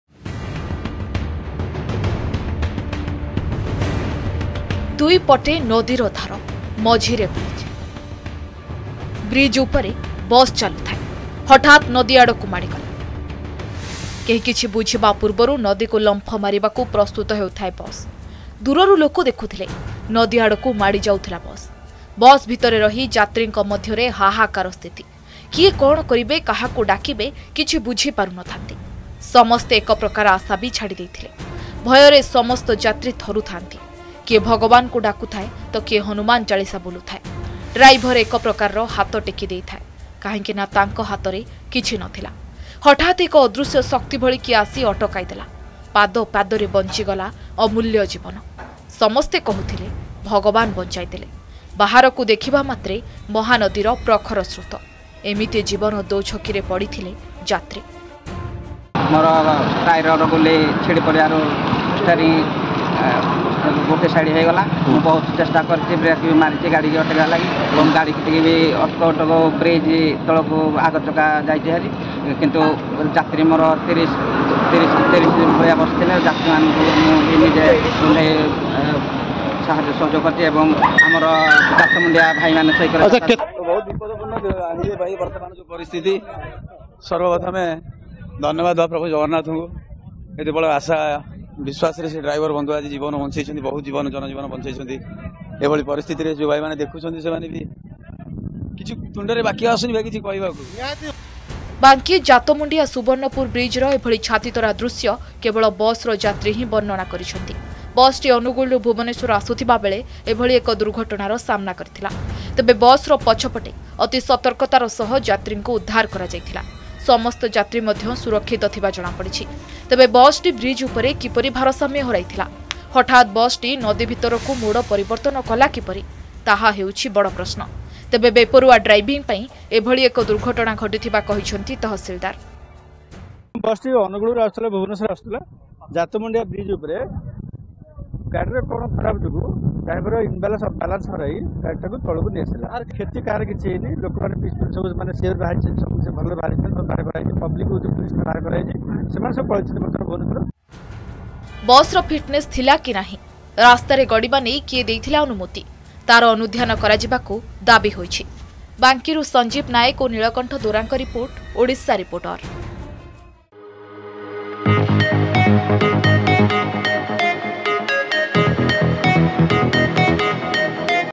ଭଗବାନ ବଞ୍ଚାଇ ଦେଲେ | Odisha Reporter
ବ୍ରିଜ ଉପରେ ବସ ଚାଲୁଥିଲା ହଠାତ ନଦୀ ଆଡକୁ ମାଡିଗଲା। ବସ ଭିତରେ ଥିବା ଯାତ୍ରୀଙ୍କ ମଧ୍ୟରେ ହାହାକାର ସ୍ଥିତି। କିଏ କଣ କରିବେ କାହାକୁ ଡାକିବେ କିଛି ବୁଝି ପାରୁ ନଥାଆନ୍ତି। ସମସ୍ତେ ଏକପ୍ରକାର ଆଶା ବି ଛାଡି ଦେଇଥିଲେ। ହଠାତ ଏକ ଅଦୃଶ୍ୟ ଶକ୍ତି ଭଳି କିଏ ଆସି ଅଟକାଇ ଦେଲା। ପାଦେ ପାଦେ ବଞ୍ଚିଗଲା ଅମୂଲ୍ୟ ଜୀବନ।